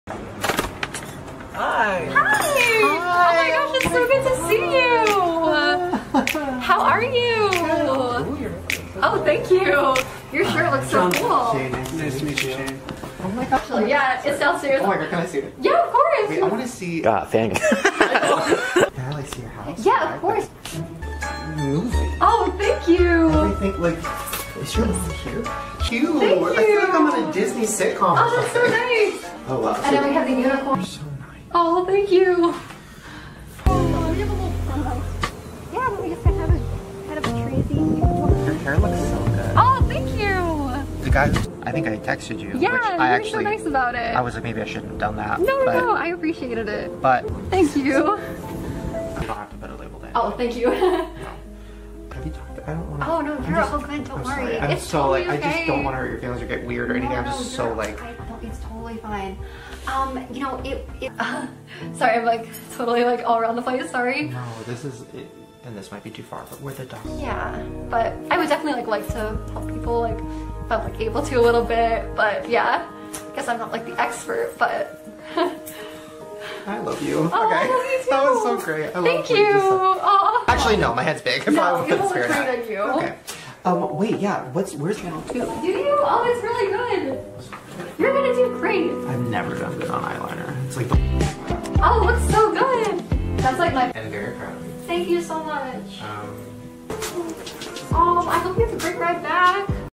EUGENIA COONEY being too nice to SHANE DAWSON for 1:39 minutes STRAIGHT!!!
Eugenia is too nice and polite sometimes, just count how many times she says thank you to Shane in this video